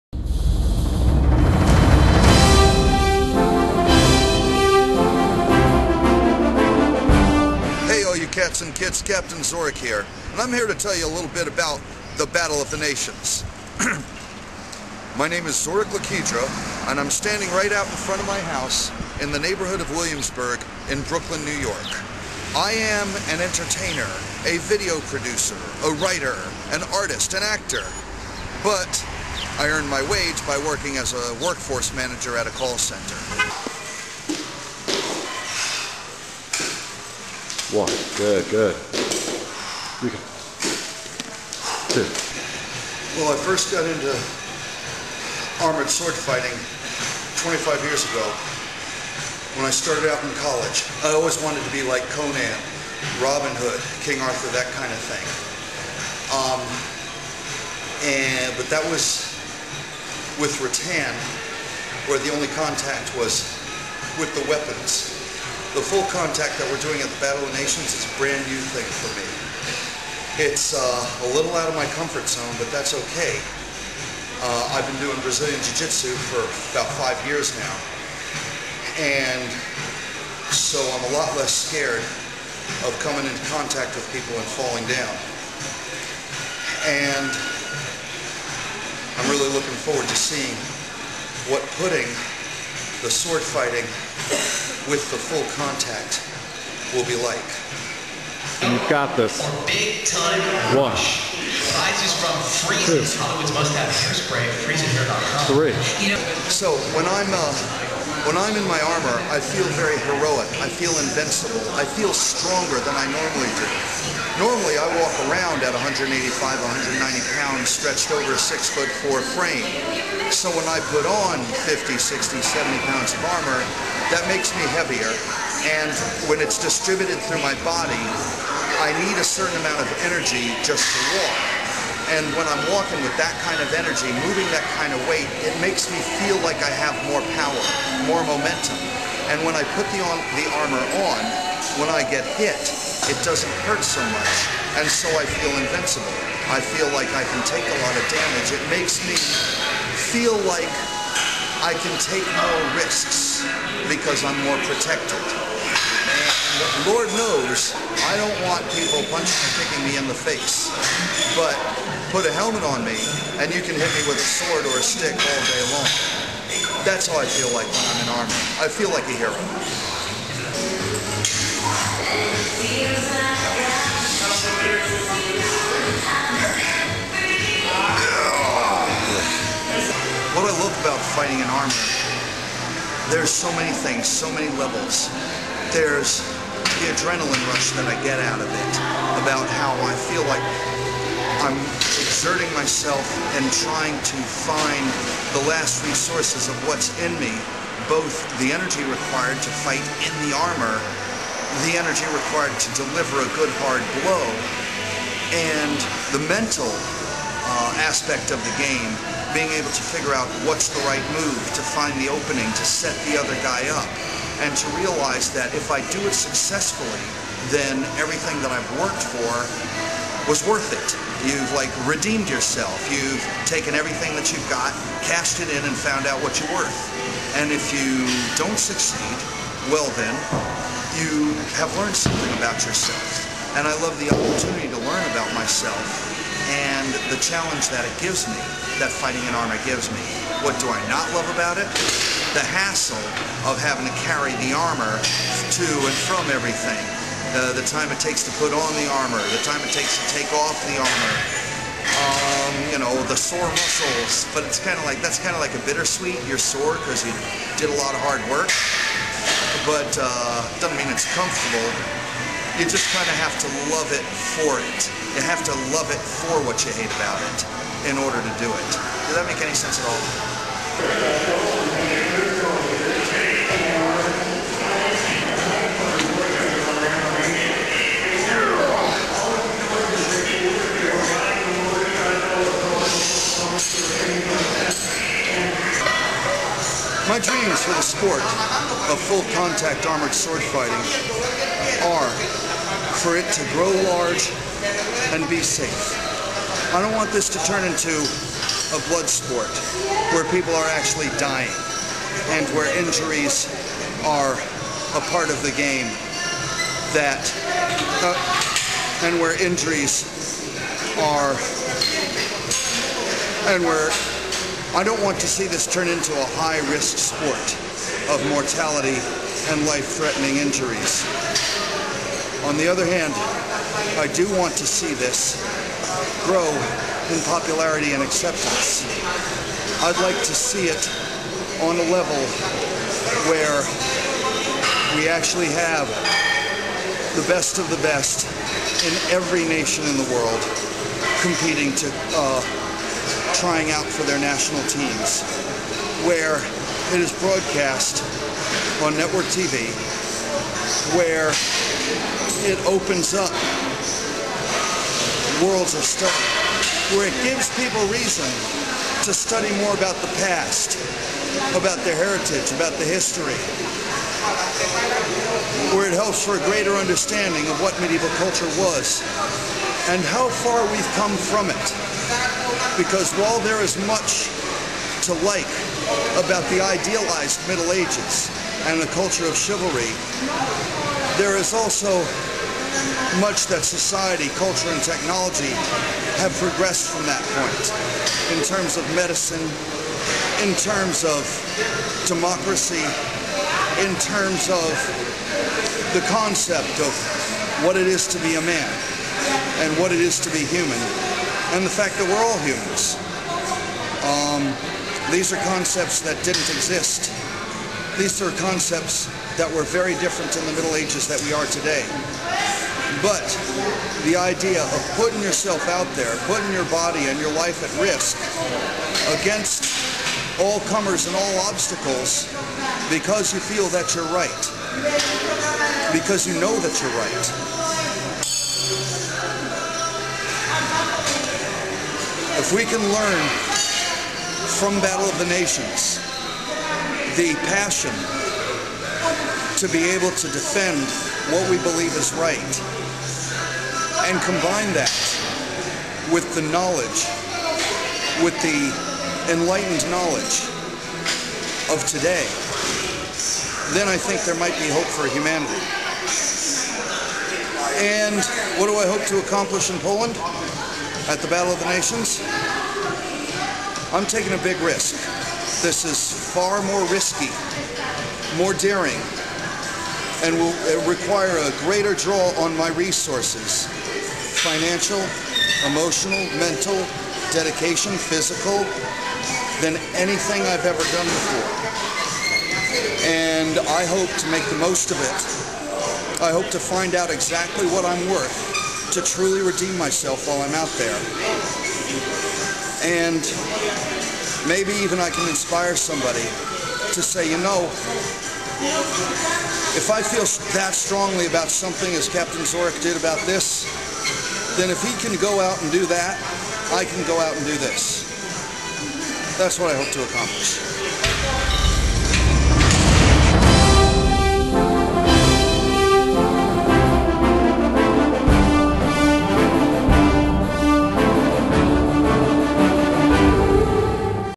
Captain Zorikh - Battle of the Nations questions
I have tried out for, and been selected to, the American team that is going to Poland this spring to compete in the Battle of the Nations, a large international medieval armored combat competition and festival. These are a series of questions put together by a video producer for members of the team to answer.

This is a journey that is demanding more out of me physically, mentally, and financially, than anything I have done. It is taking me out of my comfort zone and I will be traveling farther than I ever have to do something riskier than I ever have before. I am doing it becose I have come to love these sorts of challenges, finding out what I am worth, how far I can go. I am doing it because it is a logical step iin my martial arts/fitness quest and my journey to becoming a badass. I am also doing it to represent my country, as this is the first time there will be an American team at the Battle of the nations

You can help me on this journey and in this endeavor by visiting my Indiegogo fundraising There are special "perks" available at every level of funding!